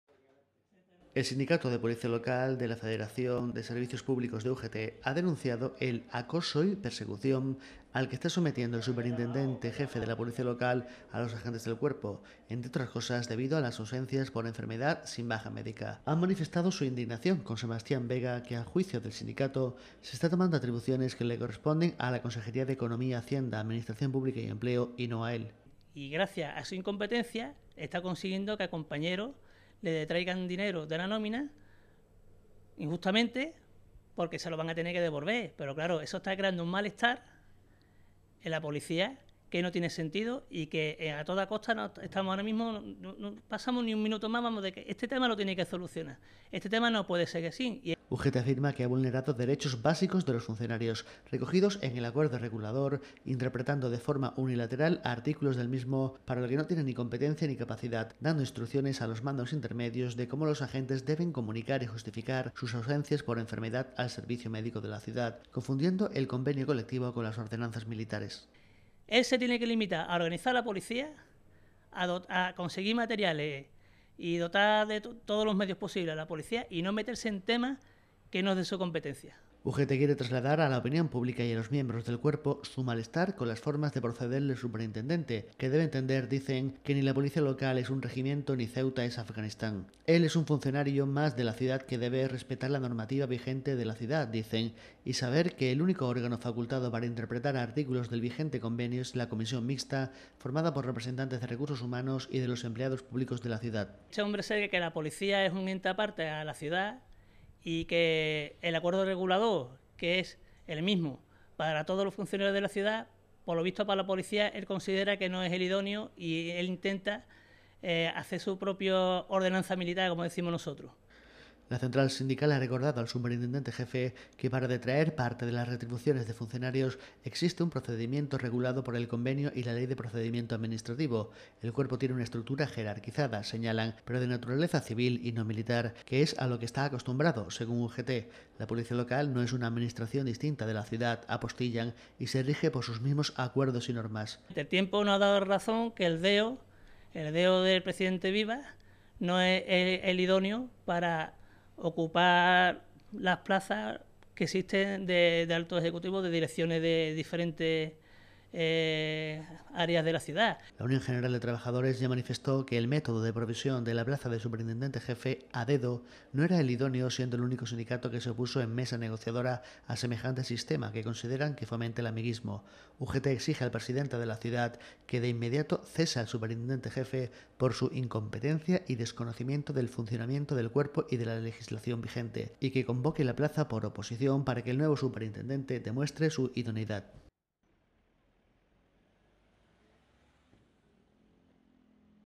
UGT denuncia “acoso y persecución” por parte del Superintendente de la Policía Loca
UGT ha exigido al Presidente de la Ciudad Autónoma que cese inmediatamente al Superintendente Jefe de la Policía Local, por considerarle incompetente y desconocedor del funcionamiento del Cuerpo. Han denunciado “acoso y persecución” por parte de Sebastián Vega a Agentes del Cuerpo, entre otras cosas, por ausencias por enfermedad sin baja médica.